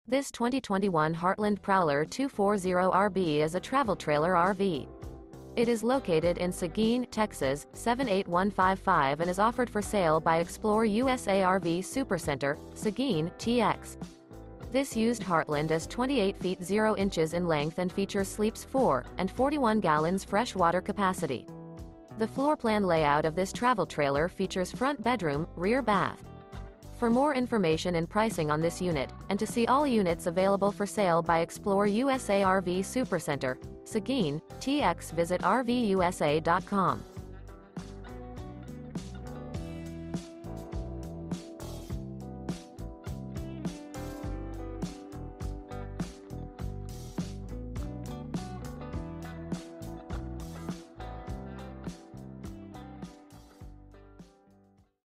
Marvelous 2021 Heartland Prowler Travel Trailer RV For Sale in Seguin, TX | RVUSA.com
This 2021 Heartland Prowler 240RB is a Travel Trailer RV. It is located in Seguin, TX 78155 and is offered for sale by ExploreUSA RV Supercenter - SEGUIN, TX. This Used Heartland Is 28' 0" in length and features sleeps 4, and 41 gal fresh water capacity. The floorplan layout of this Travel Trailer features Front Bedroom, Rear Bath.